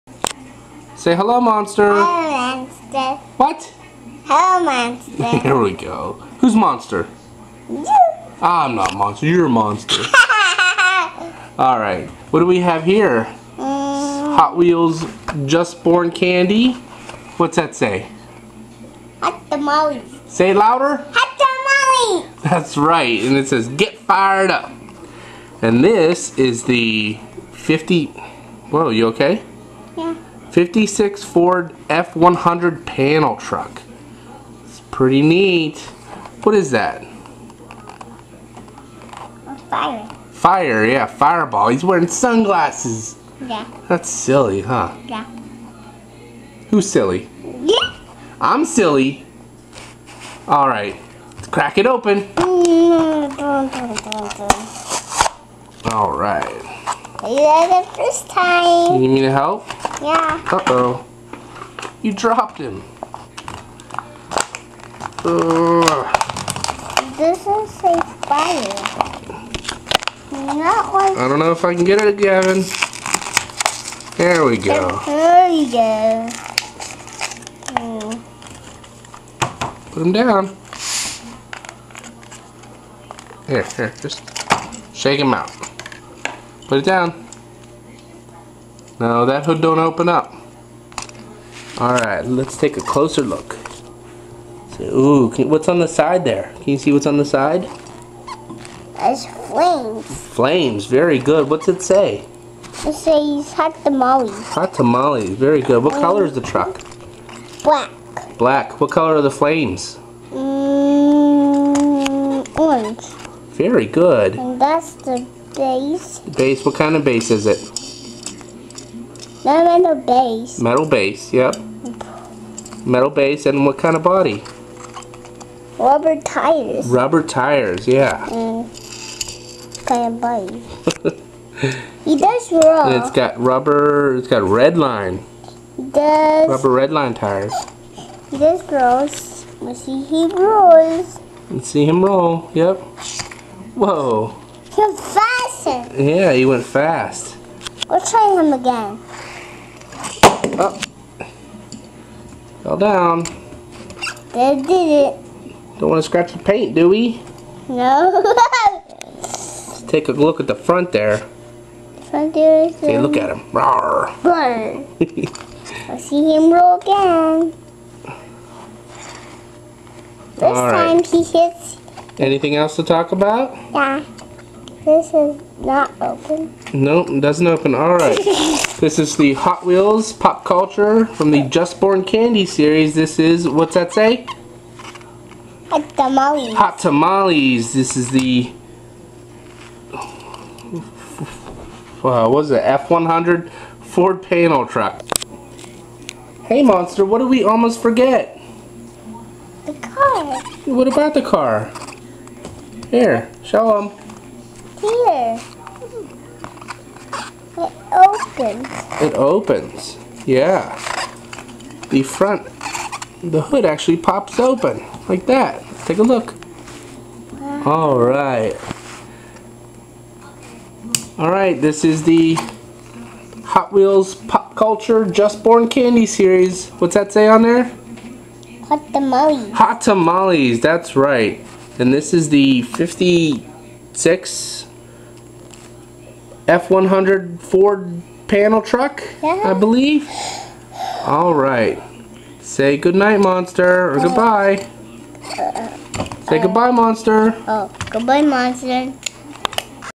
Blister Breaking #21 Hot Wheels Hot Tamales Ford F-100 Panel!!
Blister Breaking #21 Hot Wheels Just Born Candy Series Hot Tamales Ford F-100  Panel Truck!!!